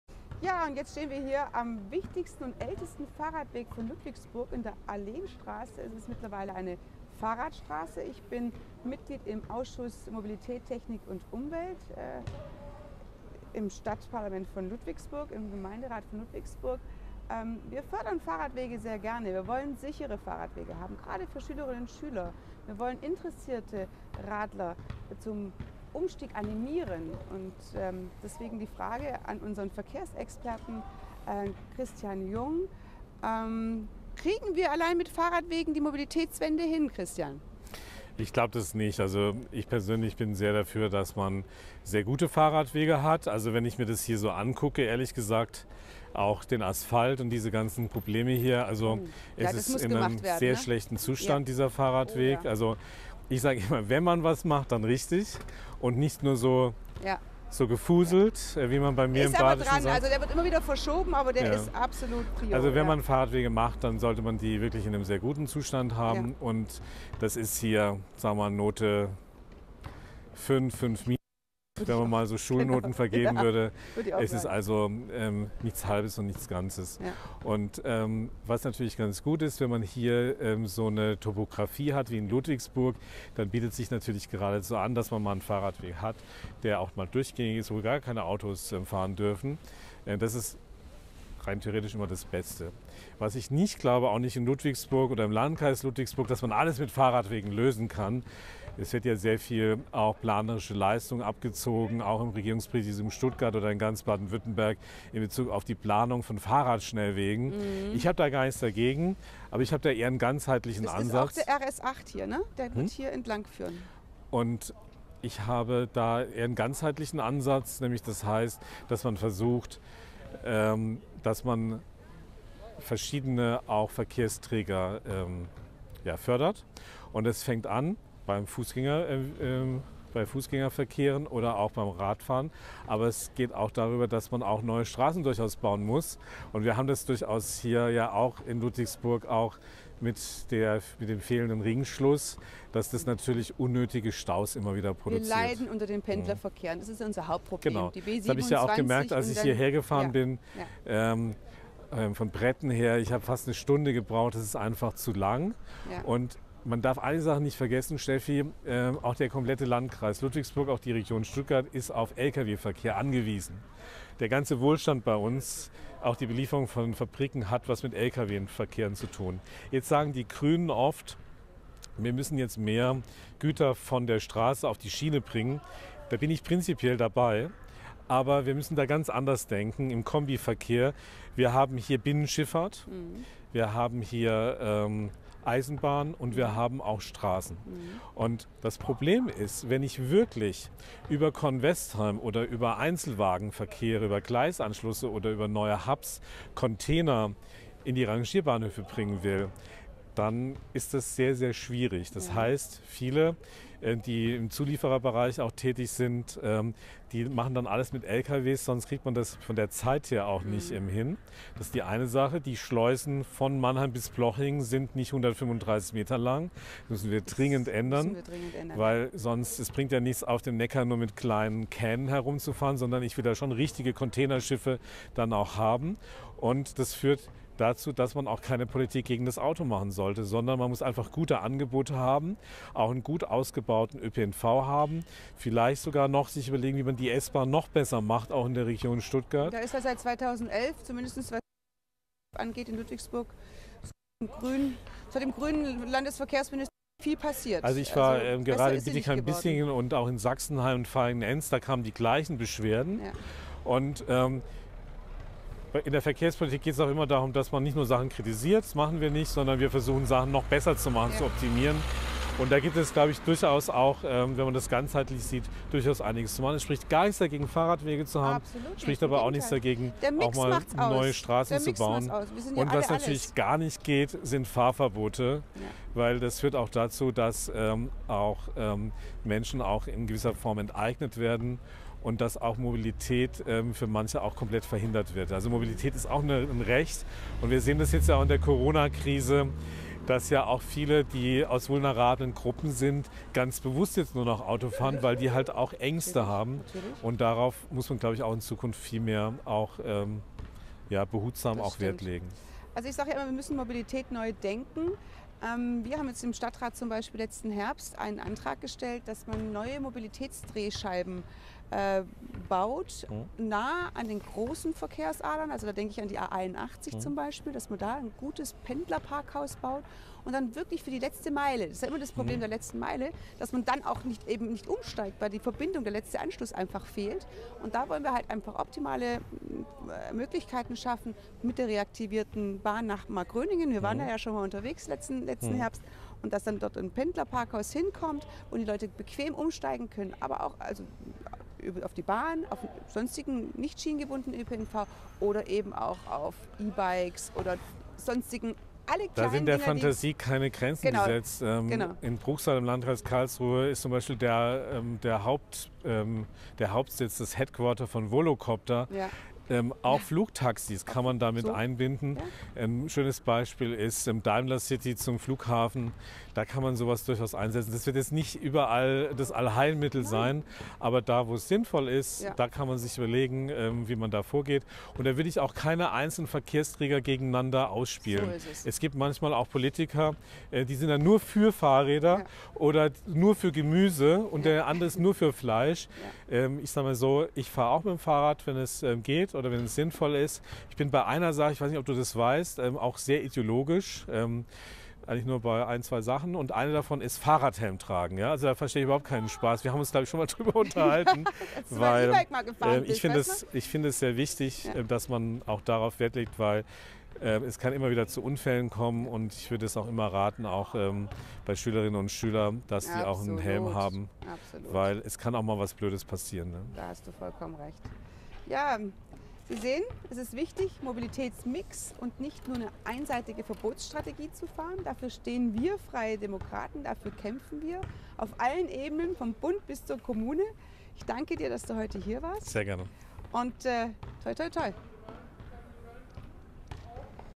Stefanie Knecht & Christian Jung: Sind Fahrradwege das Allheilmittel?
Das Gespräch mit dem FDP-Verkehrsexperten Christian Jung eröffnet die Ludwigsburger Stadträtin Stefanie Knecht mit der provokanten Frage, ob Fahrradwege das Allheilmittel für alle Verkehrsprobleme sind. Im Gespräch tauschen sich die beiden über zahlreiche Aspekte moderner Verkehrspolitik aus.